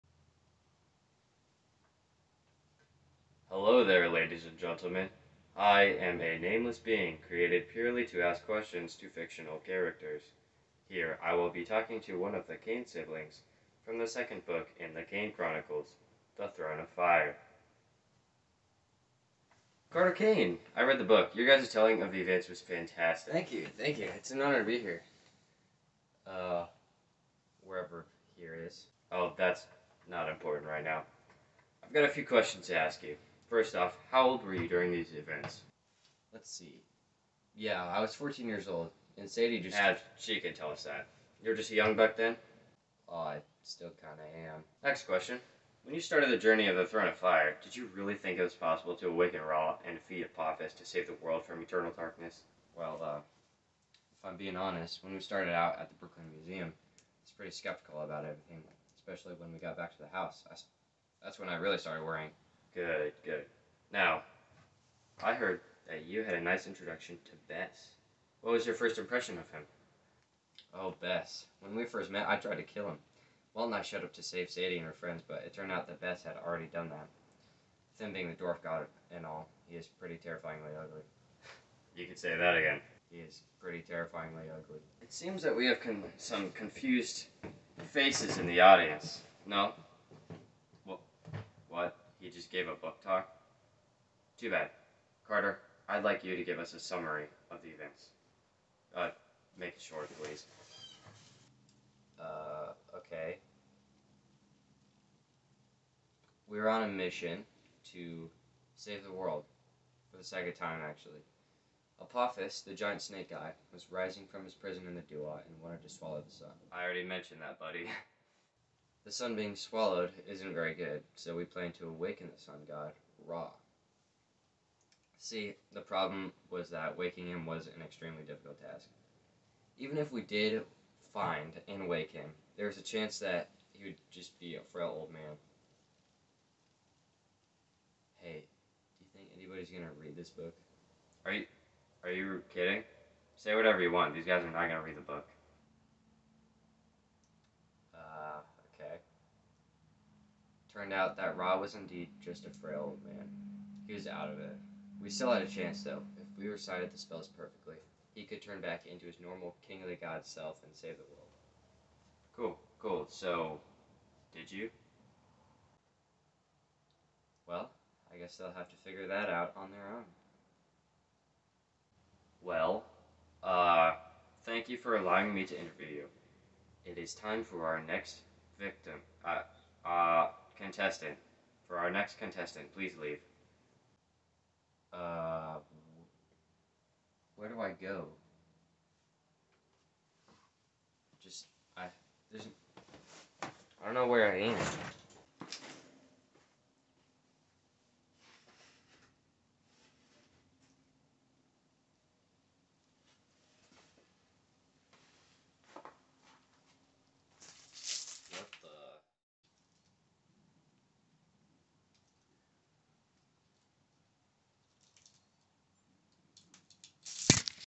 Interview with Carter Kane: The Throne of Fire
Ethan created this project on October 26, 2022 for Mr. Hundley's Honor's English 10.

He gave a book talk just before presenting this video to his class.

Carter Kane - Ethan
Interviewer - Ethan